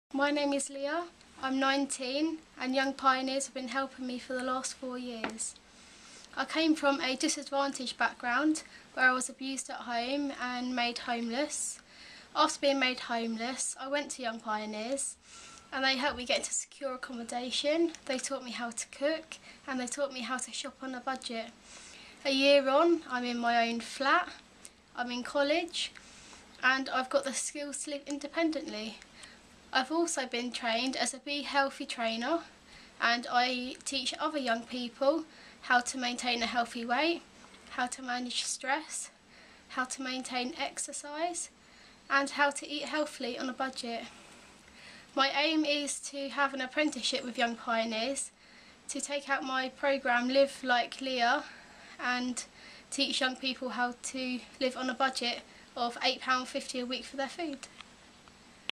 Live Like Leah
Leah a young pioneer beneficiary talks about her background, her time with young pioneers and her aspirations for the future